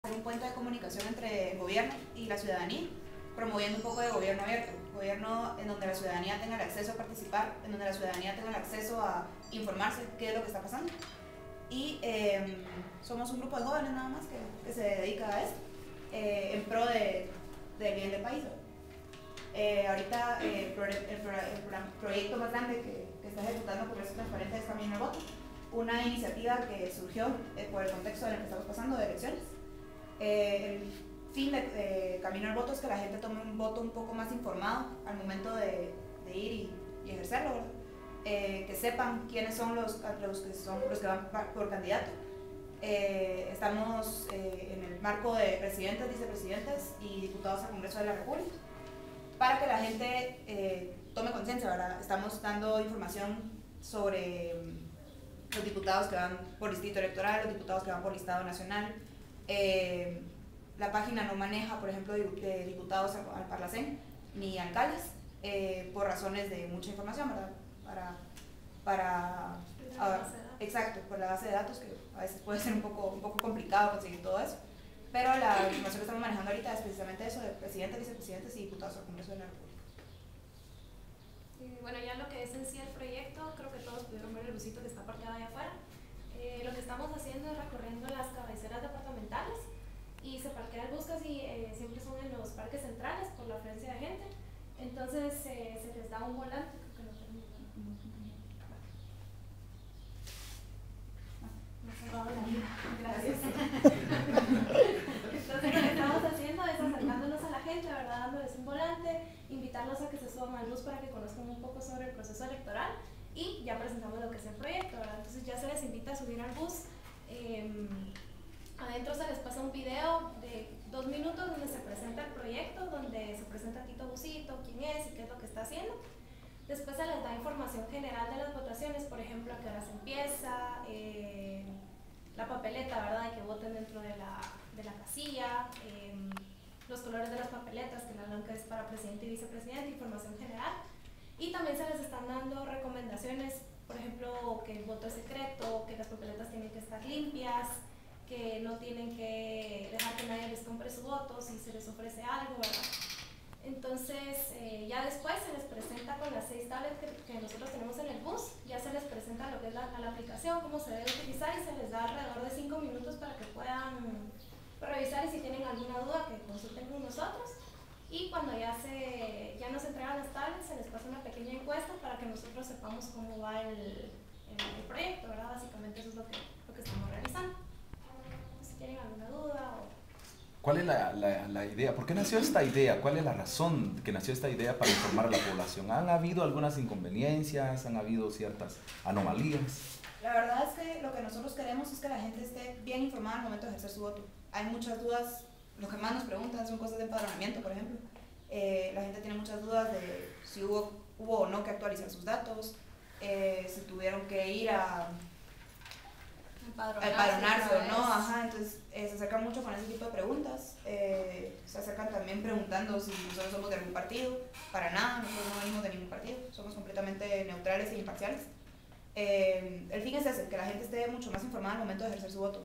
Paola y Jimena informan sobre
Patzún, Paola y Jimena informan sobre caminoalvoto el proyecto del busito interactivo para informar sobre el proceso electoral y el uso de la tecnología.